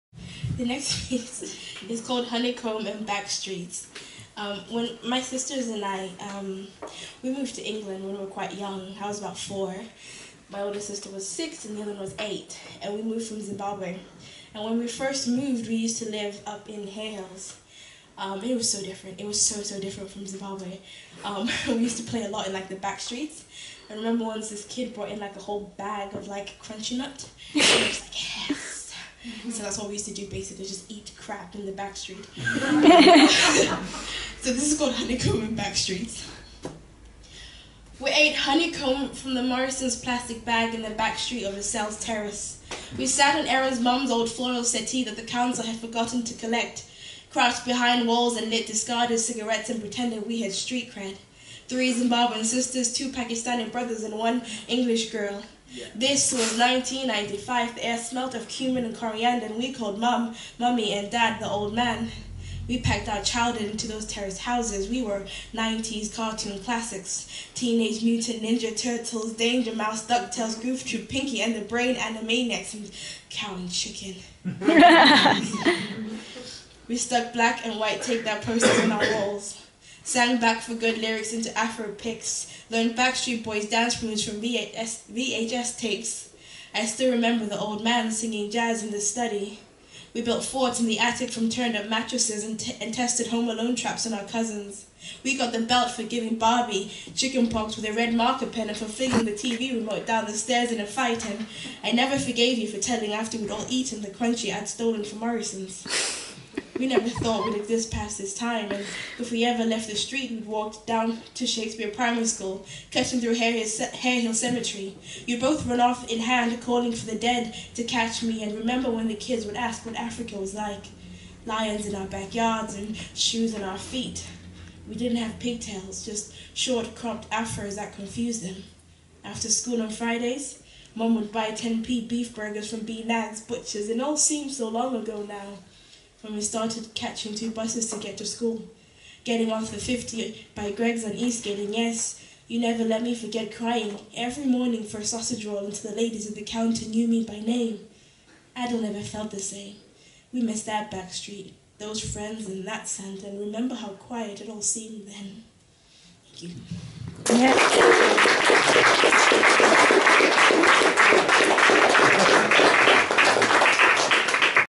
Zodwa Nyoni: Honeycomb and Backstreets Poem
Zodwa Nyoni reads her poem Honeycomb and Backstreets at DIY writers' social Fictions of Every Kind, Leeds. At Fictions of Every Kind: Home (March 2014)